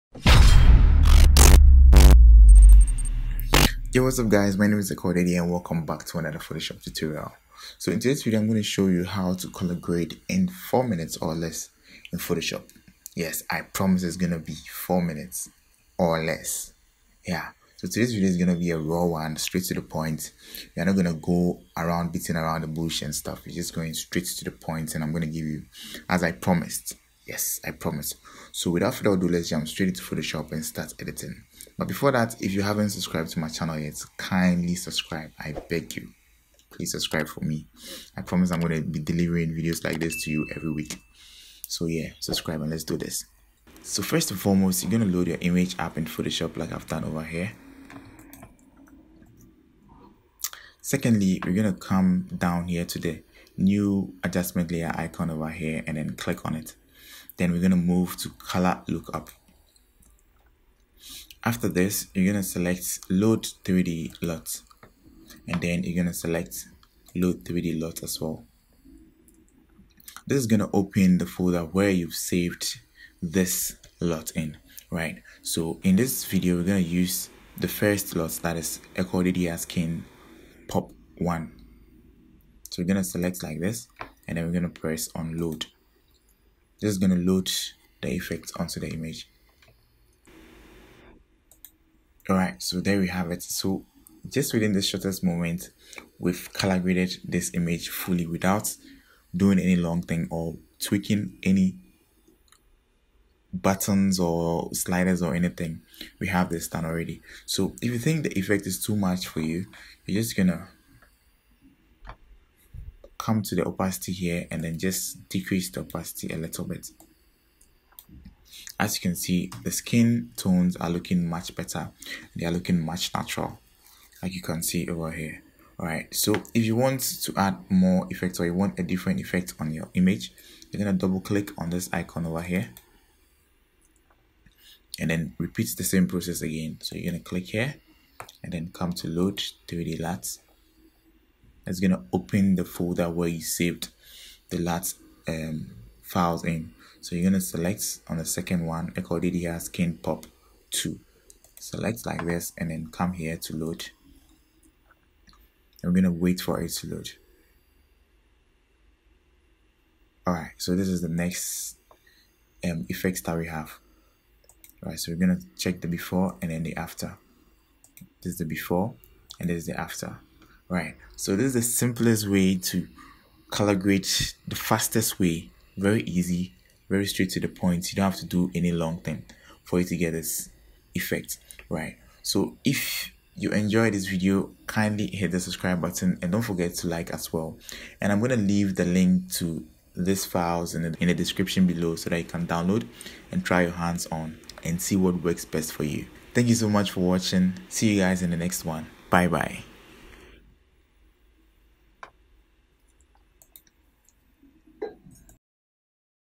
Color Grade in 4 mins or less: Photoshop Tutorial
In today's tutorial, we will learn how to quickly color grade your images in Photoshop using the power of Luts in Photoshop.

DOWNLOADS;
3. FREE LIGHTROOM AND CAMERA RAW PRESET